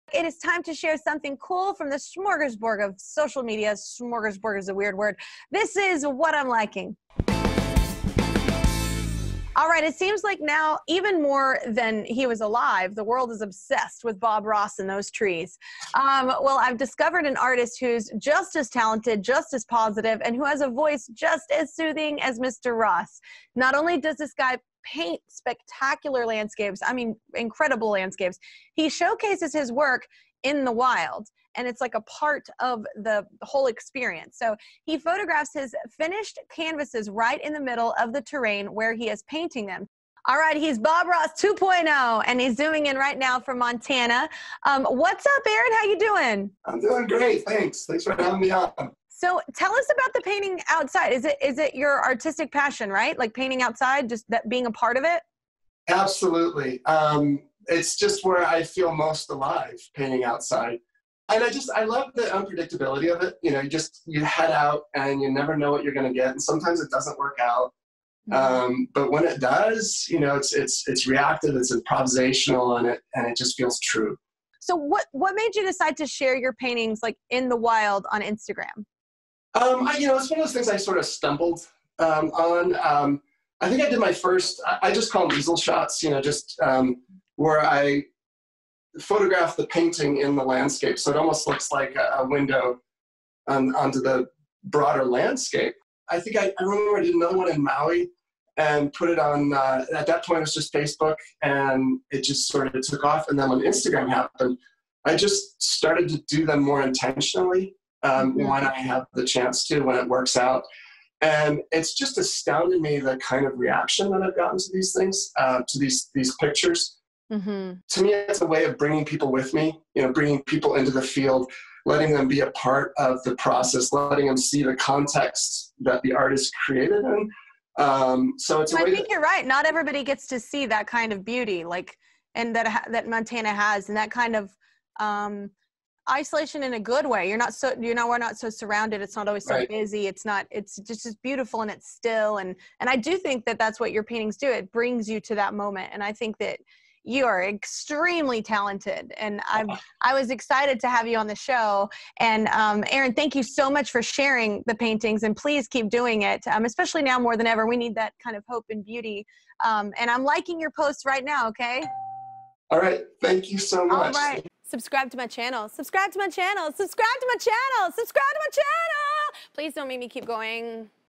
This Man's Hyper Realistic Paintings Are Impossible To Detect In Nature
Kelly Clarkson chats with Aaron Schuerr, a Montana artist whose unique landscapes dazzle fans! Aaron paints scenic backdrops of beautiful vistas, then photographs his finished painting as though it were part of the actual landscape. You have to see his stunning work to believe it!